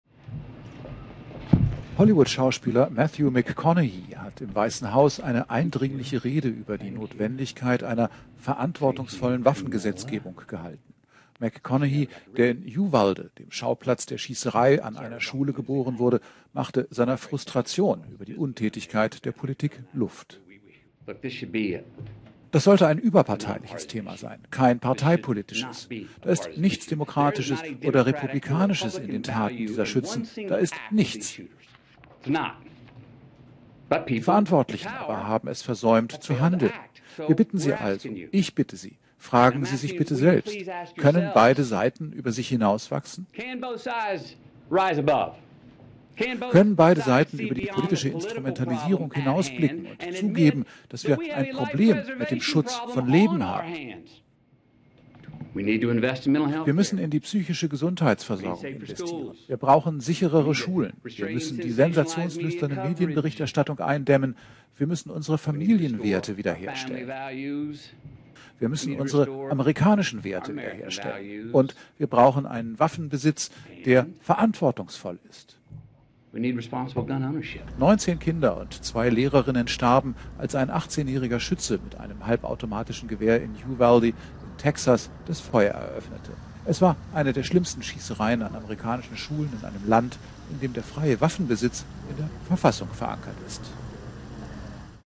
Brandrede im Weissen Haus: McConaughey zu US-Waffengesetzen
McConaughey, der in Uvalde, dem Schauplatz der Schießerei an einer Schule im vergangenen Monat geboren wurde machte seinem Frust über die Politik Luft.